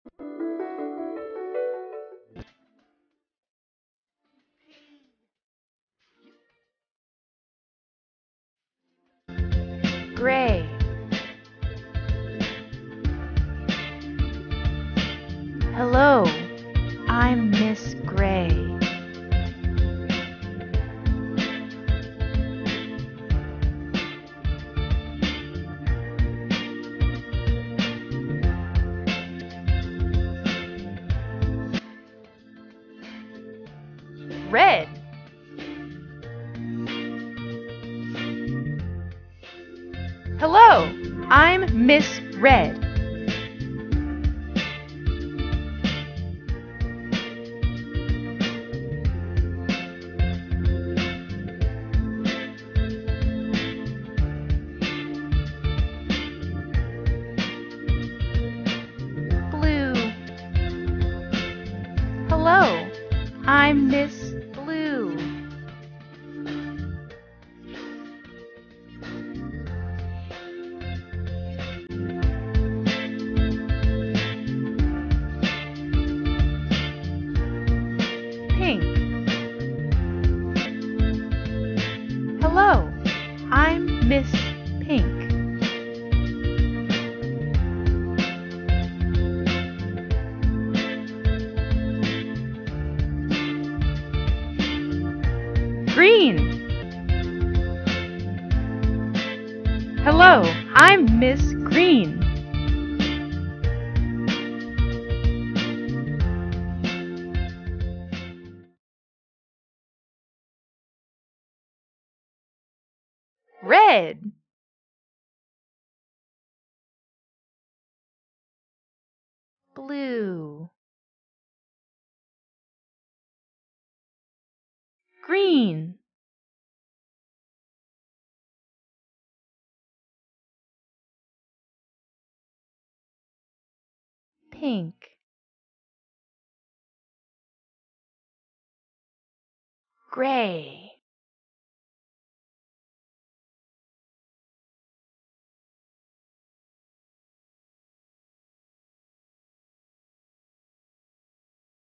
Hello Felix | Conversation Unit 6: Color - Learning english for kids
Words about different colors are represented in a creative way that helps kids in kindergarten learn English effectively.

You are watching video of Conversation Unit 6: Color on Hello Felix  - online English teaching channel for children on Youtube !!!